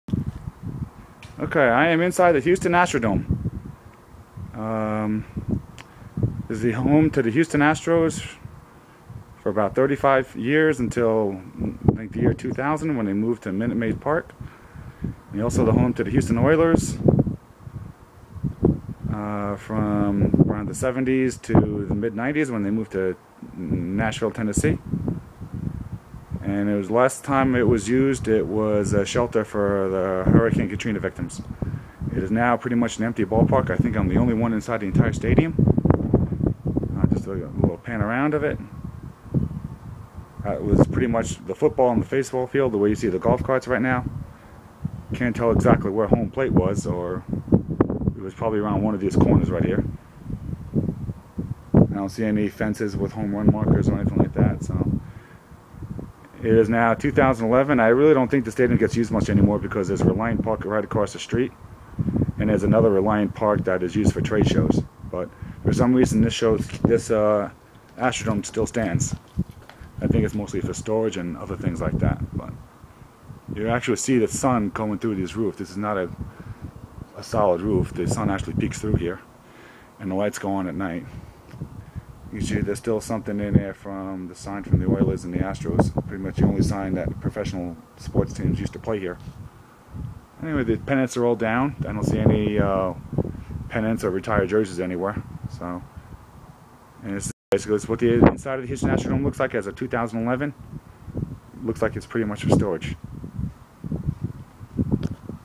Inside the Relinat Houston Astrodome in 2011.
A short video taken inside the Houston Astrodome in June of 2011, over 10 years since the Houston Astros moved to what is now Minute Maid Park, and over 15 years since the Houston Oilers moved to Tennessee. It also hosted events such as the 1992 Republican Convention, the "Battle of the Sexes" tennis match in 1973, College Basketball's "Game of the Century" between UCLA and Houston in 1968, Wrestlemania, Selena's last concert before being shout to death about two months later. It's last known use was for a shelter for Hurricane Katrina victims who migrated from New Orleans. It is almost never used for events anymore because there are two newer venues on the property of Reliant Park, one of which is the home of the Houston Texans NFL team and the other is for trade shows etc. The outside is in dire need of a power wash job and the inside is strewn with trash and the seats were filthy. This place is so neglected that I was able to gain entry through an unlocked gate and walked around for about 15 minutes uninterrupted by anyone. Probably because I appeared to be the only one in the building. I could have taken some seats home with me if I could think about how to explain why I was carrying seats with me in the parking lot. Anyway on the way out, a security guard confirmed that there were never any events at the Astrodome in the six months he has worked on the property. My understanding is this place needs millions of dollars in renovations before any events can be held, and only select employees are even allowed inside. Looks like its being used simply for storage of maintenance items used on the other facilities on the property.